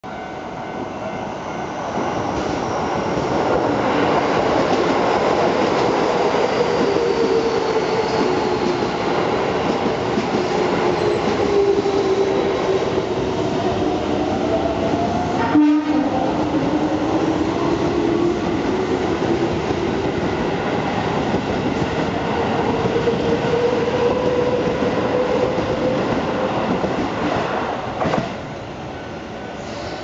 [警笛(空笛)あり]京王線 8000系8両編成 各停 千歳烏山駅発車
京王電鉄8000系8両編成京王線各停「新宿行き」の千歳烏山駅発車シーンです。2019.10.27撮影
踏切直前横断防止の為の警笛です。
京王電鉄の再生リストは下記URLからご覧下さい。約1500本取り揃えております。